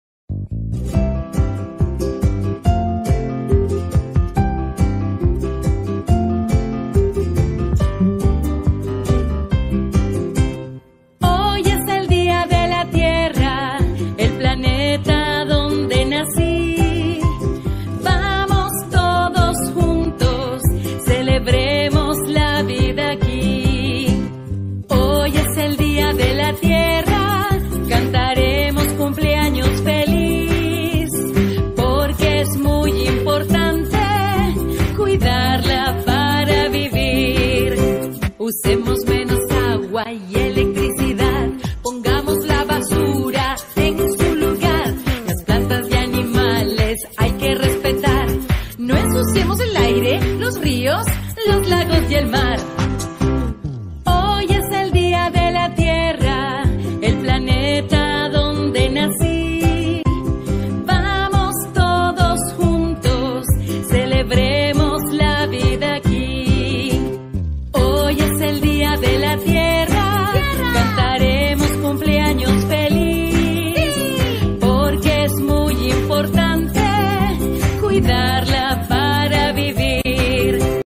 Día de la Tierra: Día Mundial de Tierra para Niños 🌎
El Día de la Tierra conocido mundialmente como el Día Internacional de la Madre Tierra, es una celebración en homenaje a nuestro planeta y se celebra anualmente el 22 de abril.